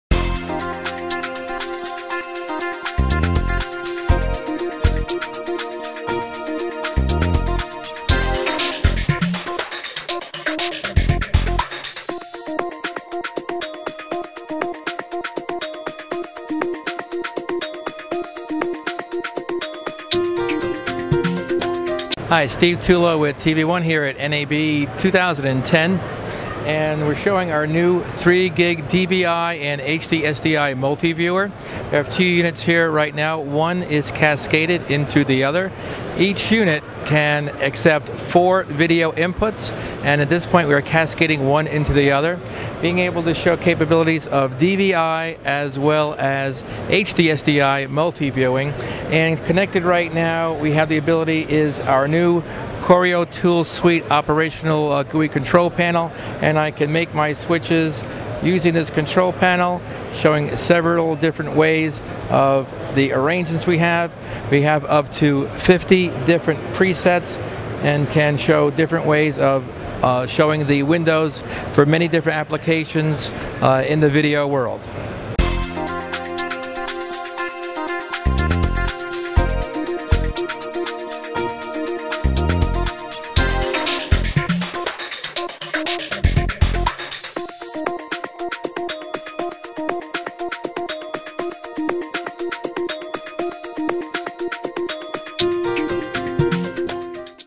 TV One C2-6104 Multi View 4-Window DVI-I Processor
CORIOview C2-6104 4-Window DVI-I Processor is based on TV One's exclusive CORIO®2 technology and has five DVI-I inputs (including one for cascade or background usage) to place up to 4 sizeable windows on a single DVI-I output. Each window is powered by a CORIO®2 scaler providing full PIP flexibility.

DVI-D resolutions up to 1920x1200@60Hz and 1080p are supported along with analog DVI-A (RGBHV/YUV/ YPbPr via optional HD-15 adaptor) resolutions up to 2048x2048 at virtually any refresh rate. Virtually all computer signal formats are accommodated. High performance de-interlacing with motion-adaptive noise reduction provides input compatibility with YUV and YPbPr sources such as 525i, 625i, 720p, 1080i and 1080p. Custom resolutions can also be added quickly and easily using available software.

All settings are stored in non-volatile memory and are retained even when power is switched off. Ten user defined presets (controllable directly from the front panel or external communications) are also available to customize settings for various applications. A built-in LCD and menu system aids quick setup. Functions can be controlled via the front panel Push Buttons, RS-232/ RS-422/RS-485 or Ethernet.

Color borders and source labeling are also provided for each window. Four Still Image Stores are built into the unit which can each display one of ten user loadable graphic images to be used in place of an input. The 5th input is available for use as a background to the 4 windows, or as a cascade input from another CORIOview unit - thus adding 4 more windows per unit.

The CORIO® Tool Suite integrates fully with the unit and most third party control systems interface directly with the entire C2 range of products.

Variable Zoom to 10X allows you to enlarge any part of the graphic image to fill the entire computer screen and position controls allow you to move around to any area desired. Variable Shrink to as little as 10% allows fitting multiple images next to each other, with full positional controls.

Advanced Features include Genlock, Chromakey, Lumakey and Mixing. The Key Mode allows any window to be keyed over any other with variable priority / layer control. The keyed image may be faded in and out. Due to the 4:4:4 sampling format, precise keying at the pixel level can be achieved.

The unit is housed in a 1RU case with rack mount ears provided and is powered from a standard 110-240v supply.